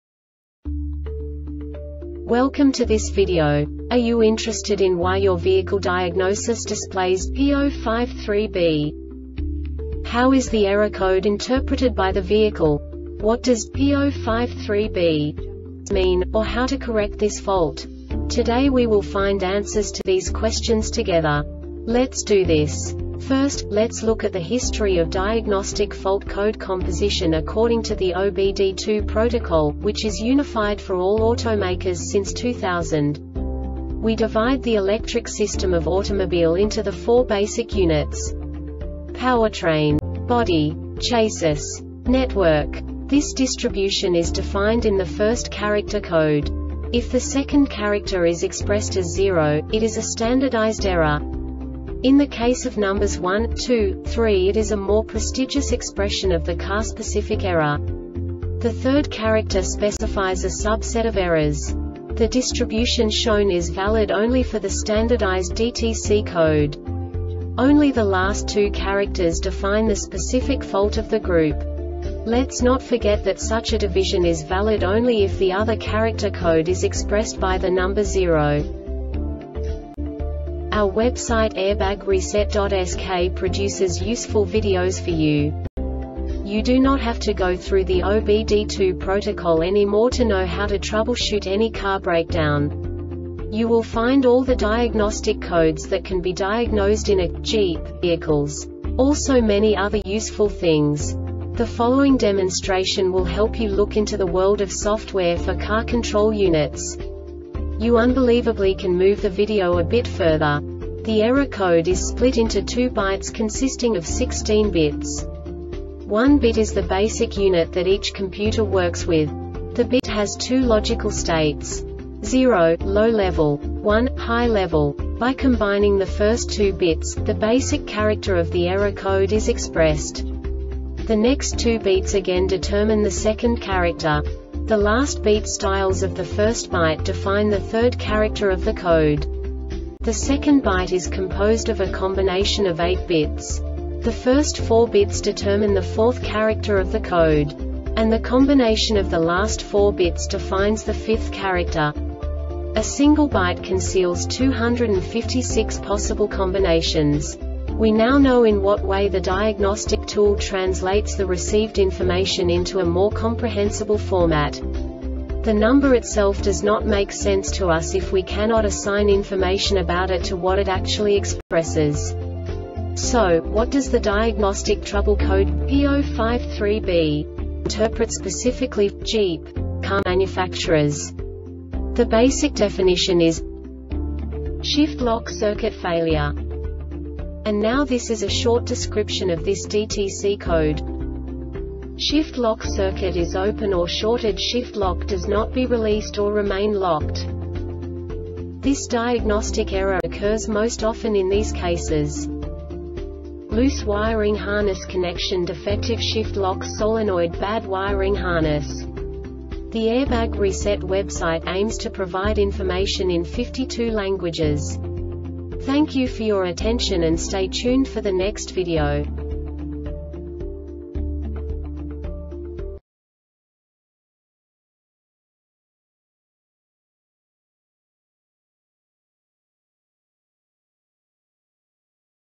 DTC Jeep P053B Short Explanation
Contents:
0:21 Basic DTC analysis according to OBD2 protocol standard.
1:48 Insight into programming
2:58 A diagnostically understandable description of the Diagnostic Trouble Code
3:05 Basic error definition

Make:
Jeep
Code:
P053B
Definition:
Shift Lock Circuit Failure
Description:
Shift lock circuit is open or shorted.Shift lock does not be released or remain locked.
Cause:
Loose wiring harness connectionDefective shift lock solenoidBad wiring harness